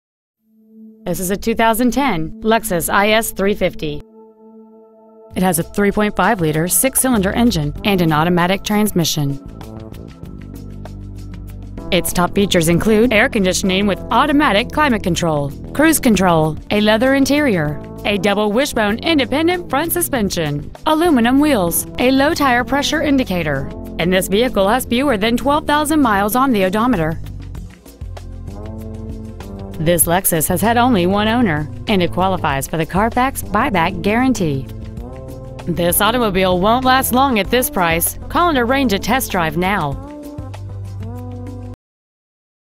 2010 Lexus IS 350C Carrollton TX 75006
We are proud to present this 2010 Lexus IS 350C .

 Year : 2010

 Make : Lexus

 Model : IS 350C

 Engine : 3.5L DOHC 24-valve V6 engine

 Trans . : Automatic

 Exterior : Gray

 Miles : 11,802

 Interior : Black

 eCarOne

 1875 N. Interstate 35E

 2010 Lexus IS 350C Carrollton TX